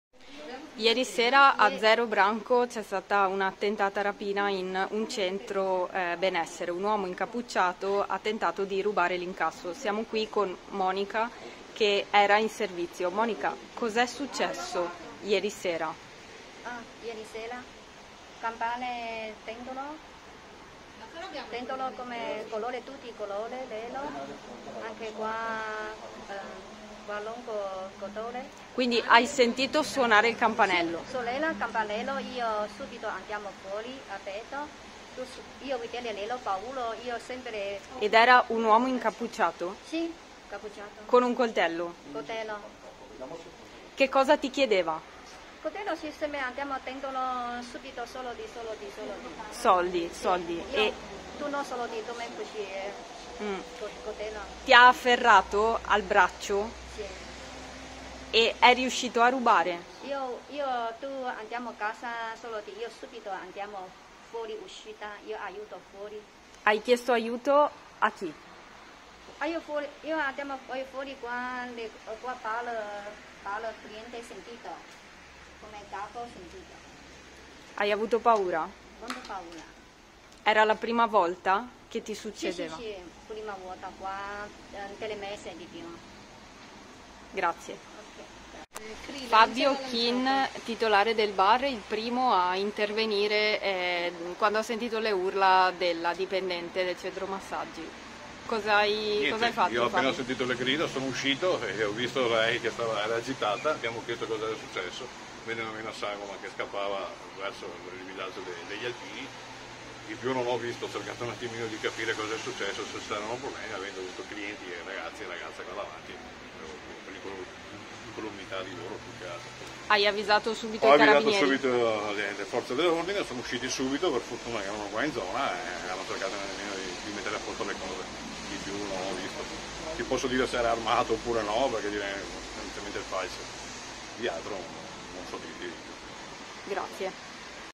Bandito travisato tenta la rapina al centro benessere di Zero Branco
Videointervista di Maria Elena Pattaro